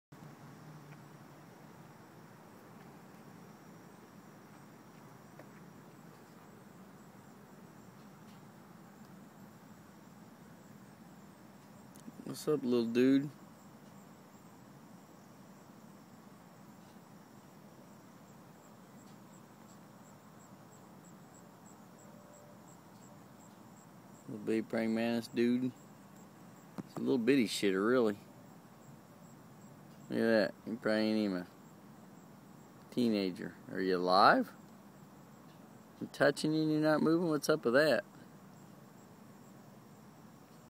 Baby mantis on my truck
Baby mantis was on the grill of my truck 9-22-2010